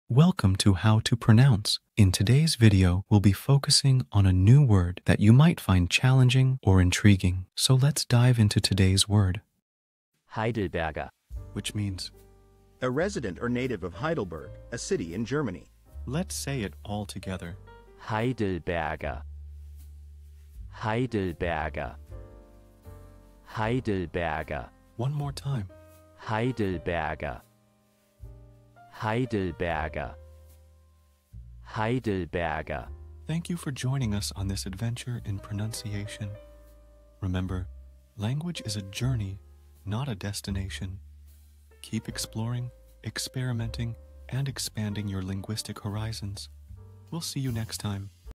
How to pronounce Heidelberger
🌍 Learn a new language now Master the Pronunciation of 'Heidelberger which Means Heidelberger' - which means : A resident or native of Heidelberg, A City in Germany. 📝 with @HTPronounce 🎓🗣️ - [Your Guide to German]

Learn to pronounce 'Heidelberger' perfectly in German with our easy and engaging tutorial by @HTPronounce! 📖📽️ In this comprehensive video, we provide you with native speaker examples 🗣️👂 and phonetic breakdowns 🎵 to ensure you get the pronunciation just right.

How to say Heidelberger in German